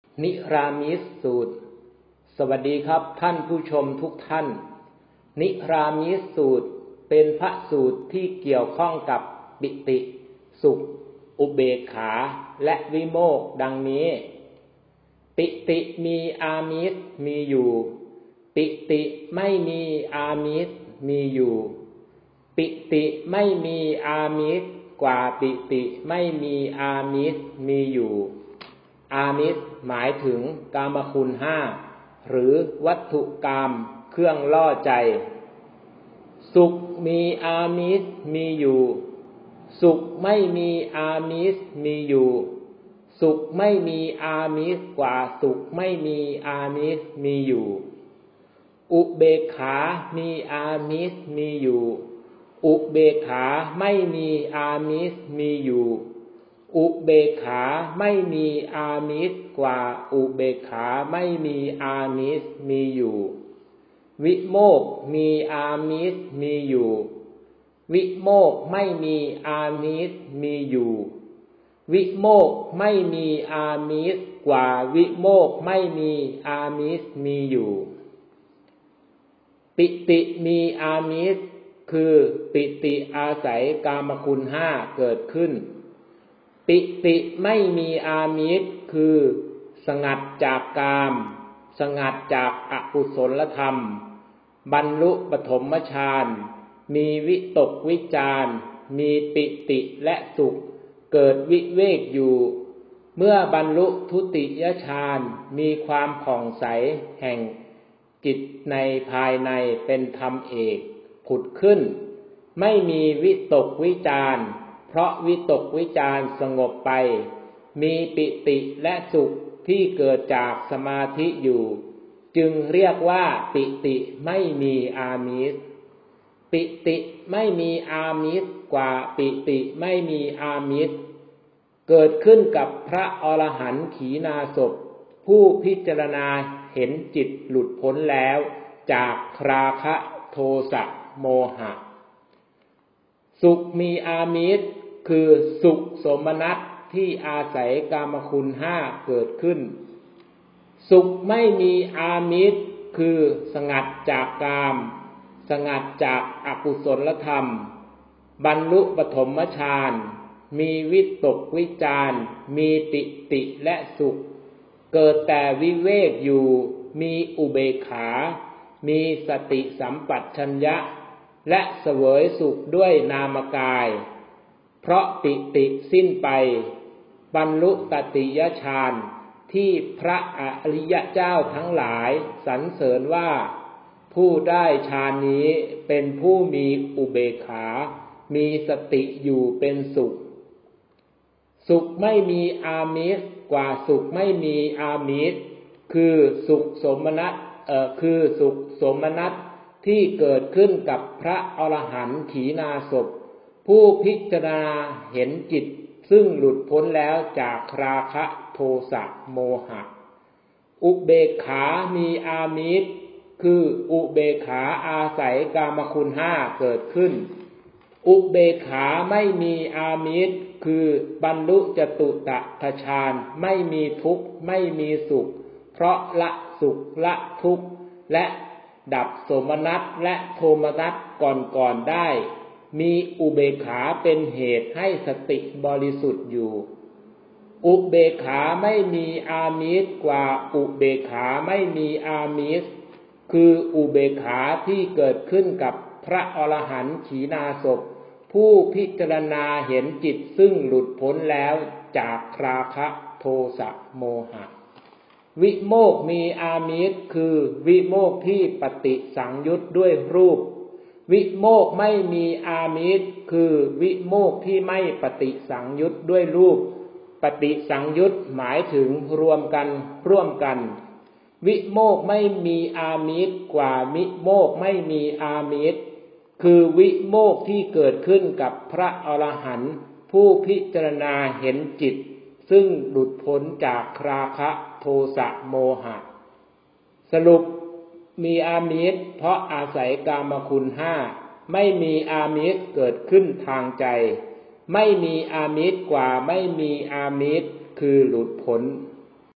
นิรามิสสูตร การพิจารณา ปิติ สุข อุเบกขา และวิโมกข์
ศึกษาไว้เป็นแนวทาง
ในการปฏิบัติธรรม กันนะครับ

นิรามิสสูตร
เป็นพระสูตร ที่เกี่ยวข้องกับการพิจารณา ปิติ สุข อุเบกขา และวิโมกข์
โดยการพิจารณา เป็นองค์ ไปทีละองค์
ว่า มีอามิส(กามคุณ5) เป็นอย่างไร
ไม่มีอามิส เป็นอย่างไร
ไม่มีอามิสกว่าไม่มีอามิส เป็นอย่างไร

ขอให้มีความสุข ความสบายใจ
ในการพิจารณาธรรม กันนะครับ